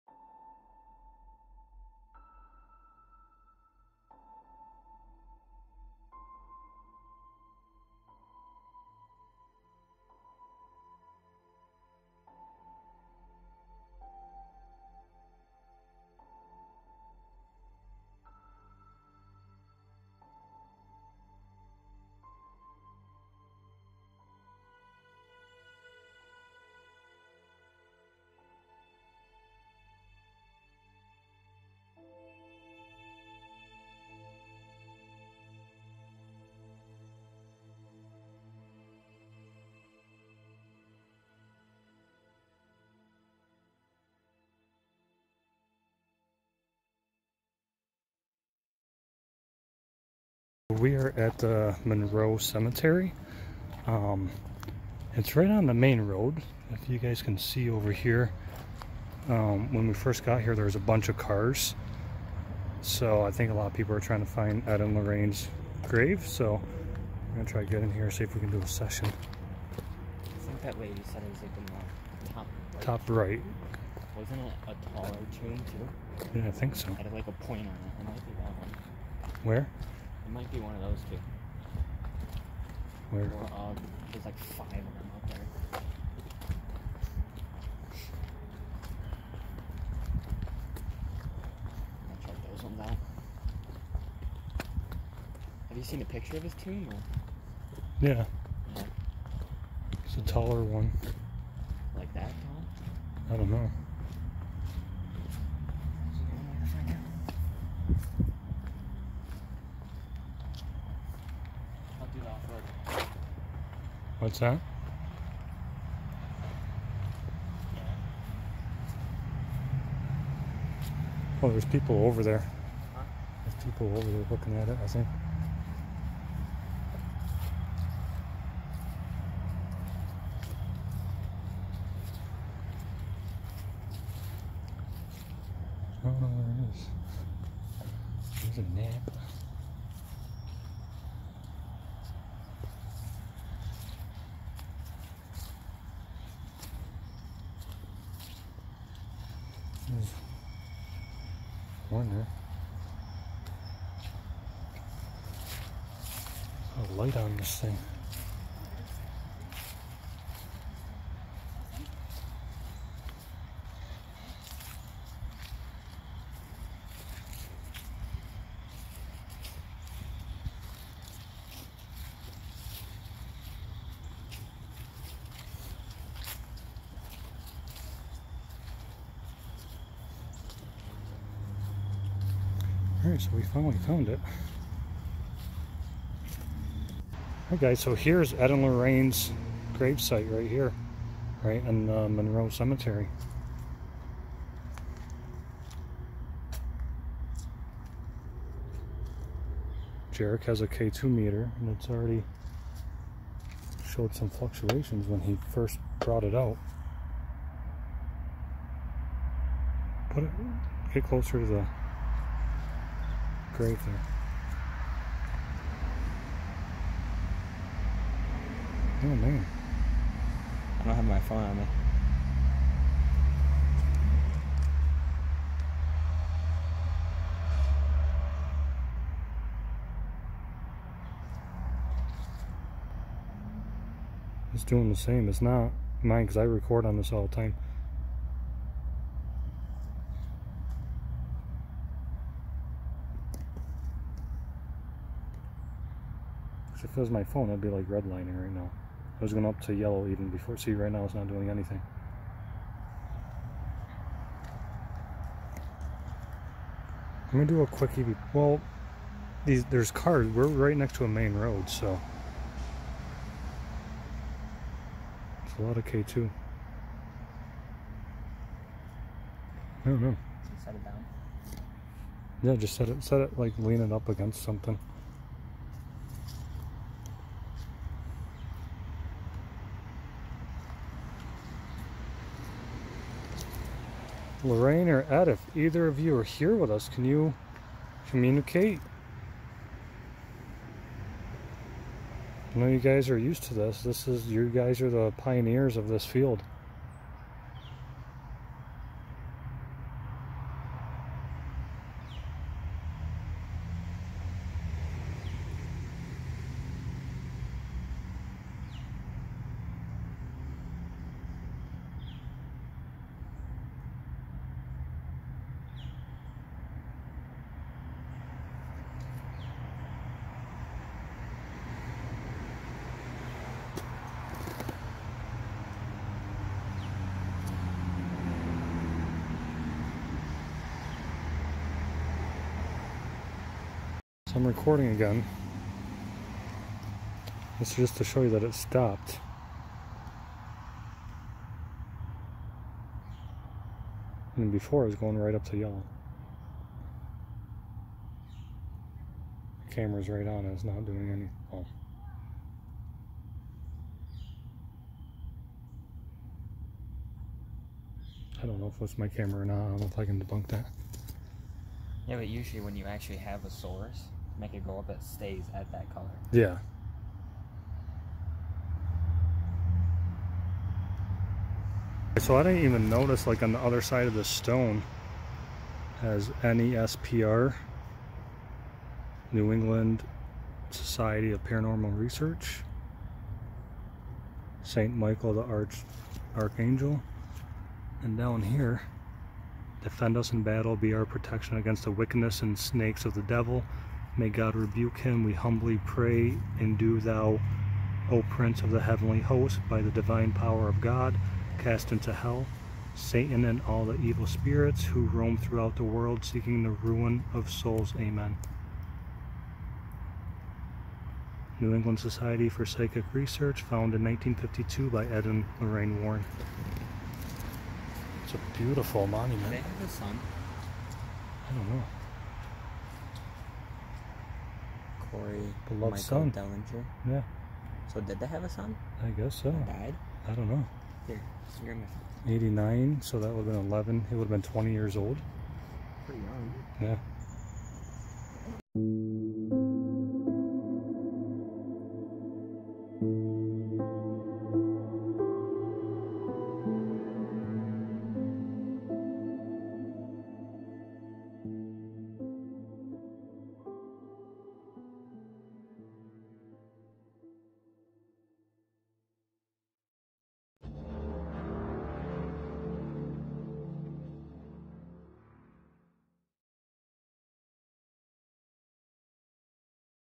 Visiting the gravesite of Ed and Lorraine Warren!
We visit Stepney Cemetery in Monroe, Connecticut to pay a visit to the gravesite of the famed duo Ed and Lorraine Warren.

The pair famously investigated the Amityvill Haunting, the Perron (The Conjuring) Hauntings, the Annabelle Case, the Arnie Johnson case (The Devil made me do it case, Conjuring 3)  and the Enfield poltergeist (Conjuring 2).